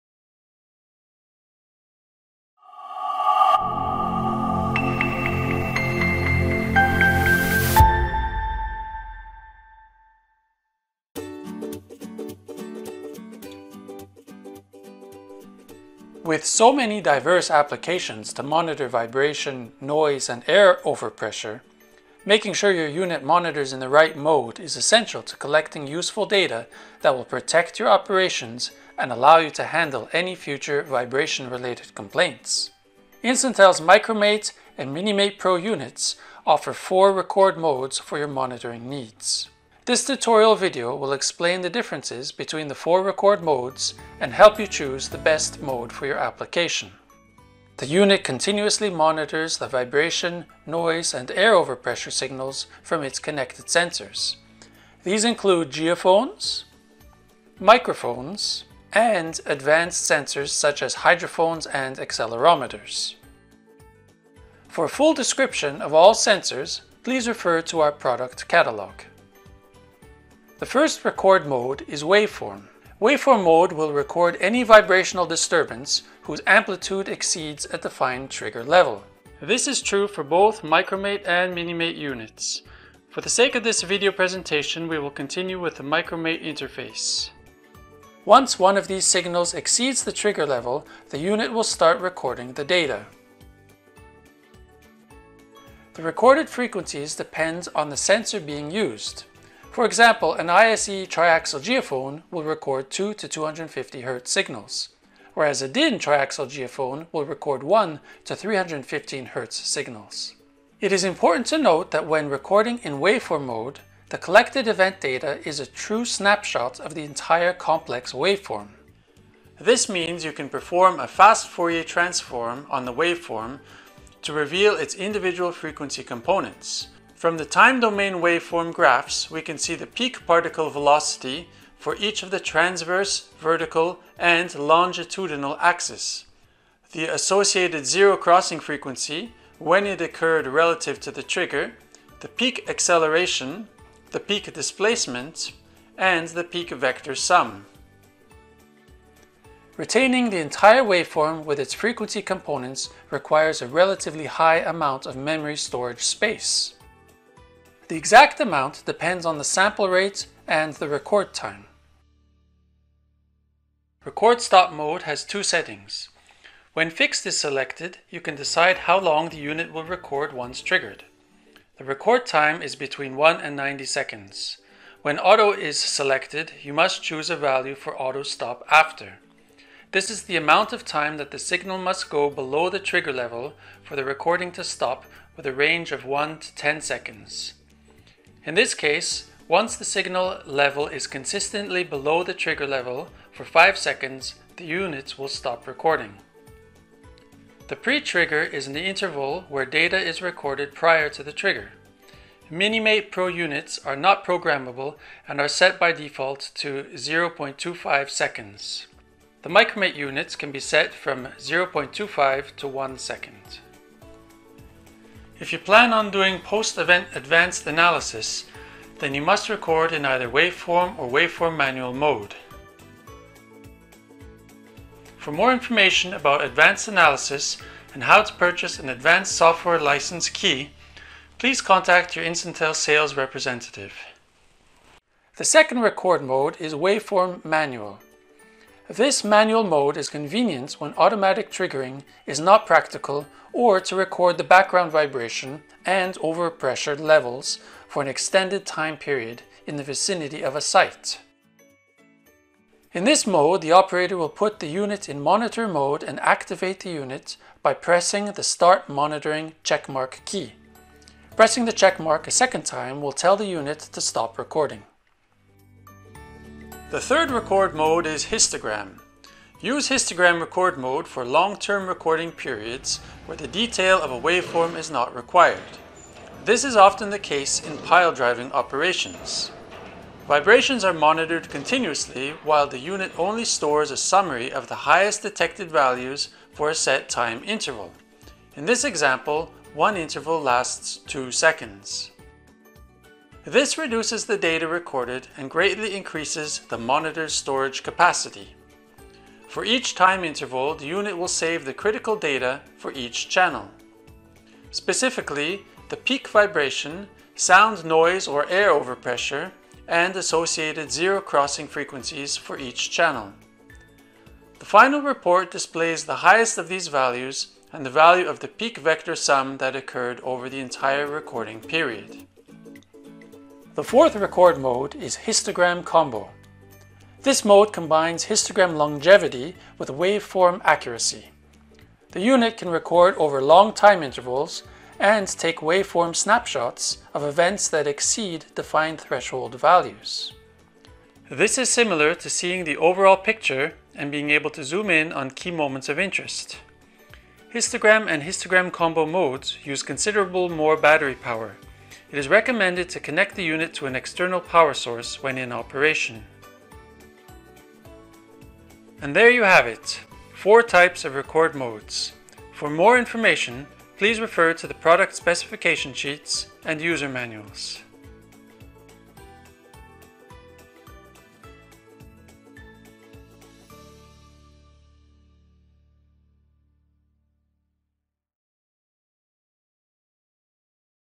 Which Monitoring Mode Should I Use_2023?
With so many diverse applications to monitor vibration noise and air overpressure, making sure your unit monitors in the right mode is essential to collecting useful data that will protect your operations and allow you to handle any future vibration related complaints.

Instantel’s Micromate and Minimate Pro units offer four record modes for your monitoring needs. This tutorial video will explain the differences between the 4 record modes, and help you choose the best mode for your application.